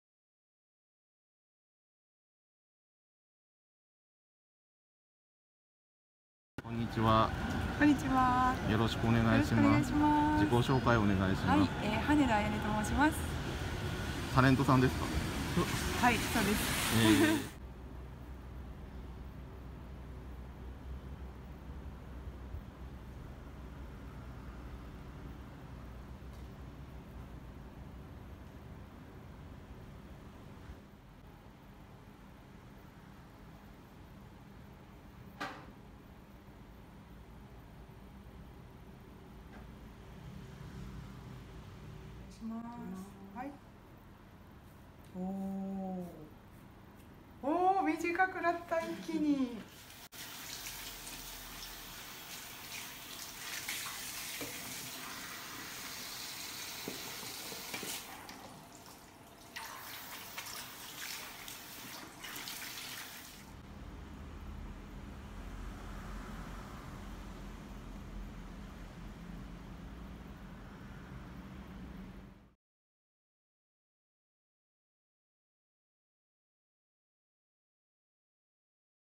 現役モデルの美しい黒髪ロングをバッサリ断髪しました。A Japanese beauty chopped her beautiful hair off for hair donation.
モデルとしてご活躍中の羽田彩音様が美しい黒髪ロングをヘアドネーションの為に約3０cmバッサリ短くされました。

撮影にご協力して頂ける美容室様、理容室様も募集中です。

A Japanese beauty chopped her beautiful hair off about 30cm for hair donation.